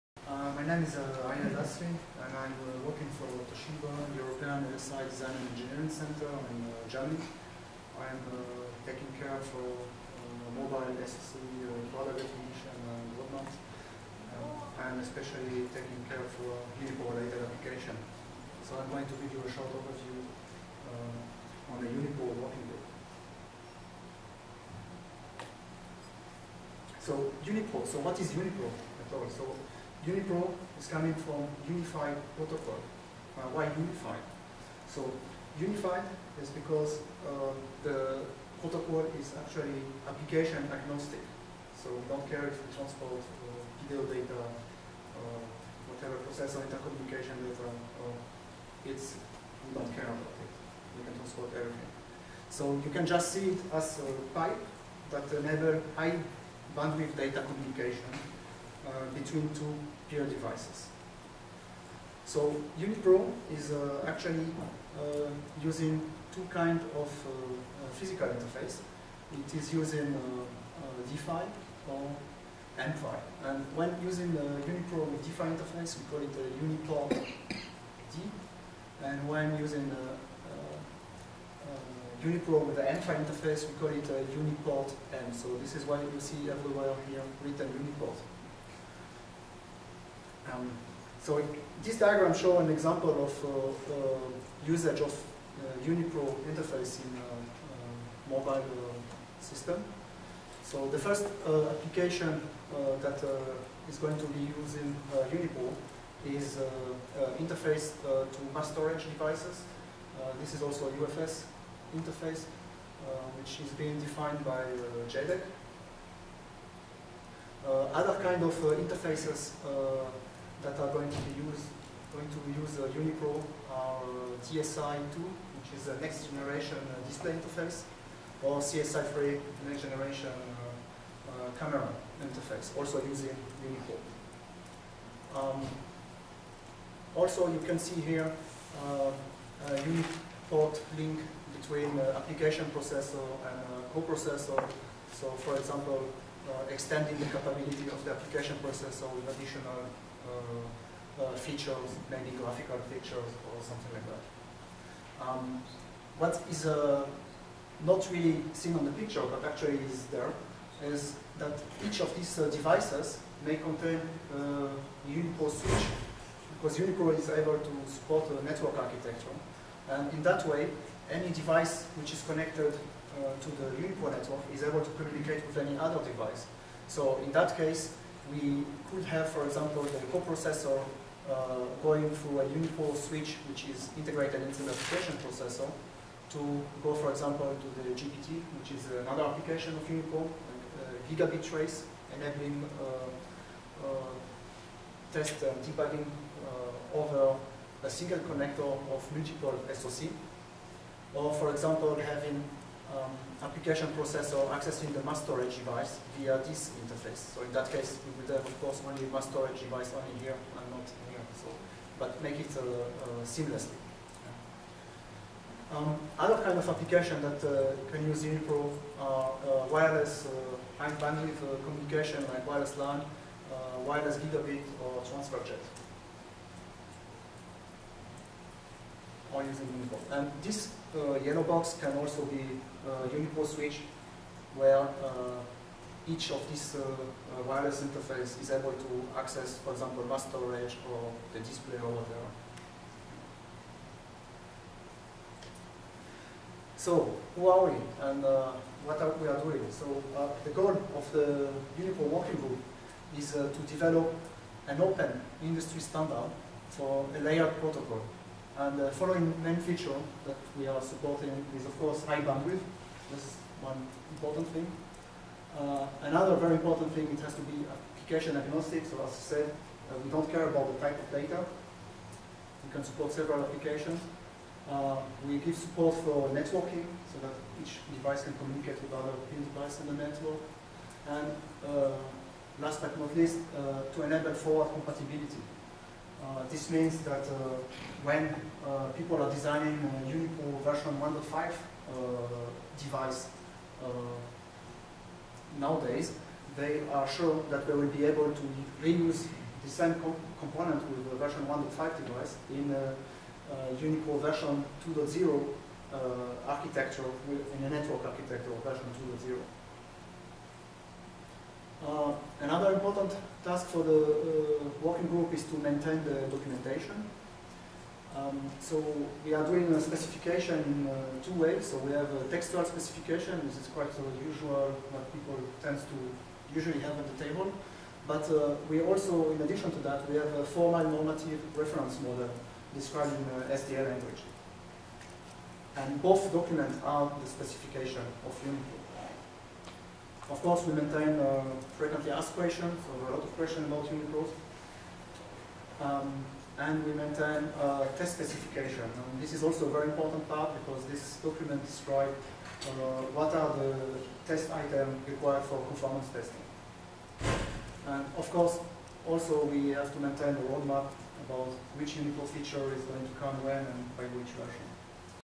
MIPI Alliance UniPro WG Overview (1/2)
Ariel Lasry of Toshiba provides an overview of the MIPI Alliance UniPro Working Group.

The MIPI Alliance Standard for UniPro (Unified Protocol) defines a layered protocol for interconnecting devices and components within mobile systems such as cellular telephones, handheld computers, digital cameras, etc. UniPro allows these devices and components to exchange data at high data rates, at low pin counts and at low energy per transferred bit. A suite of general-purpose features aims to make UniPro applicable for a wide range of component types ( e.g. application processors, coprocessors, modems, peripherals) and different types of data traffic (e.g., control messages, bulk data transfer, packetized streaming).

The UniPro WG is currently developing the UniPro 1 point-to-point specification as well as the PIE (Processor Interface Emulation) specifications. Future developments in the UniPro WG will target support for a network of devices, including UniPro 1 endpoints.